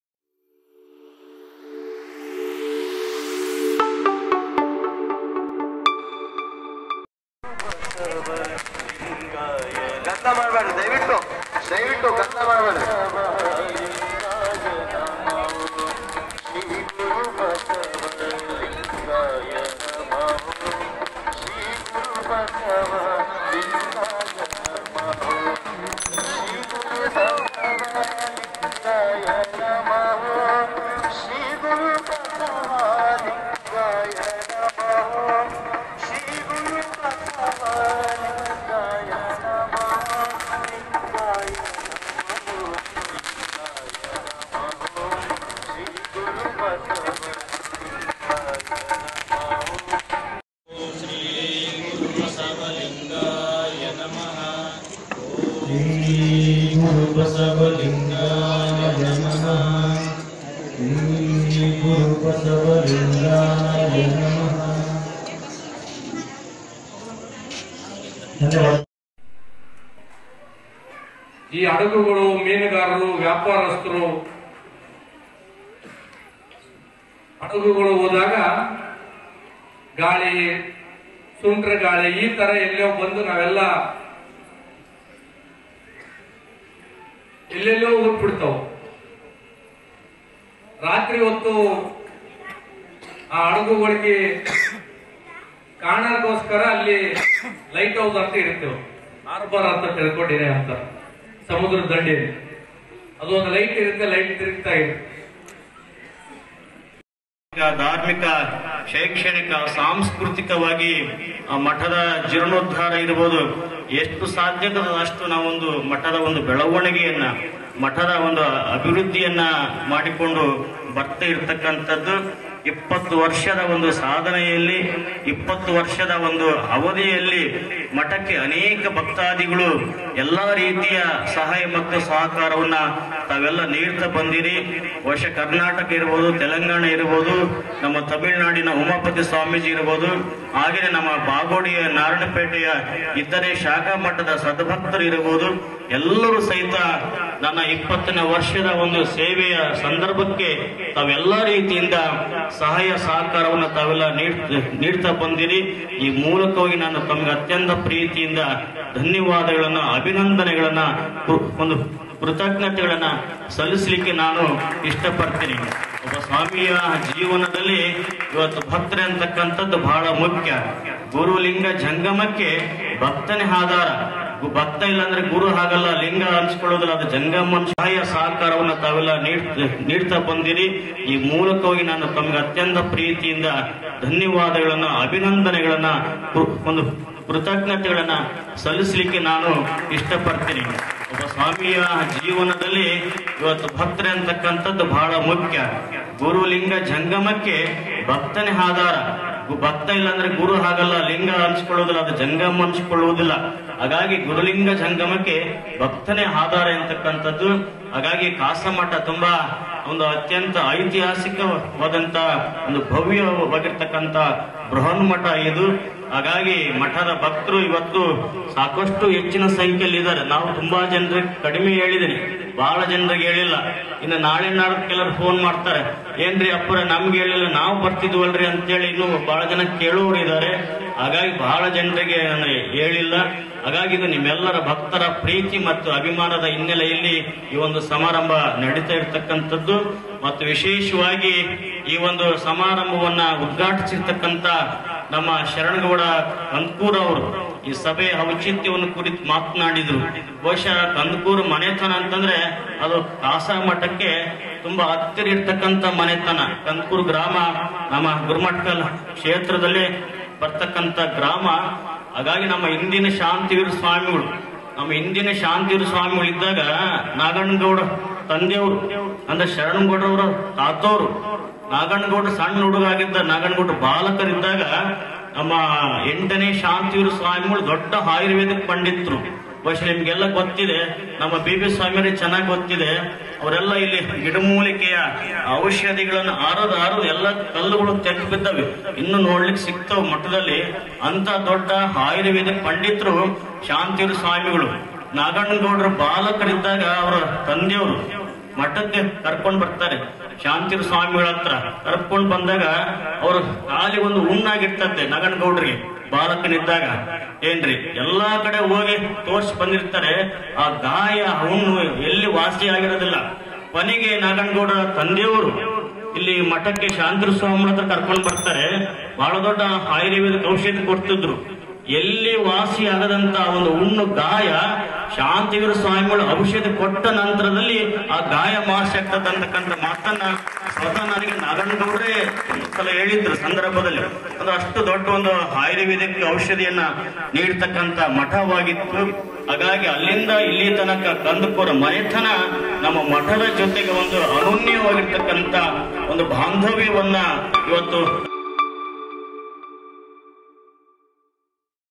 ದಶಮಾನೋತ್ಸವ ಸಮಾರಂಭ
ಗುರುಮಠಕಲ್: ಇಲ್ಲಿನ ಖಾಸಮಠದ ಪೂಜ್ಯ ಶಾಂತವೀರ ಗುರು ಮುರುಘರಾಜೇಂದ್ರ ಮಹಾಸ್ವಾಮಿಗಳವರ ಪಟ್ಟಾಧಿಕಾರದ ದ್ವಿತೀಯ ದಶಮಾನೋತ್ಸವ  ಸಂಭ್ರಮ ಅದ್ದೂರಿಯಾಗಿ ಜರುಗಿತು.